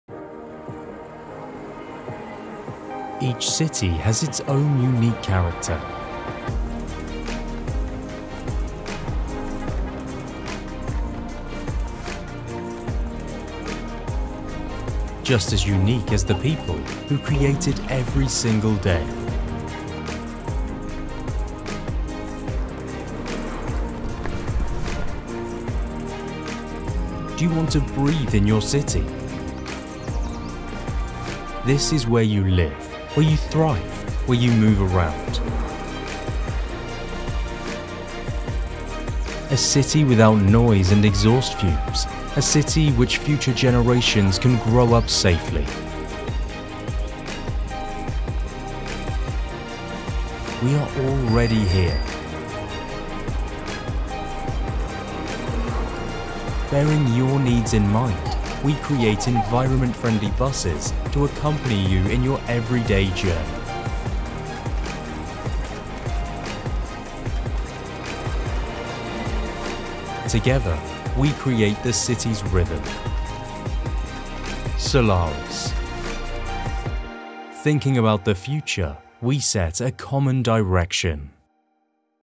Solaris - Common Direction 2
Each city has its own unique character. Just as unique as the people, who create it every single day.

Do you want to breathe in your city? This is where you live, where you thrive, where you move around. A city without noise and exhaust fumes, a city in which future generations can grow up safely. We are already here. Bearing your needs in mind we create environment friendly buses to accompany you in your everyday journey. Together we create the city’s rhythm.

Solaris. Thinking about the future we set a COMMON DIRECTION.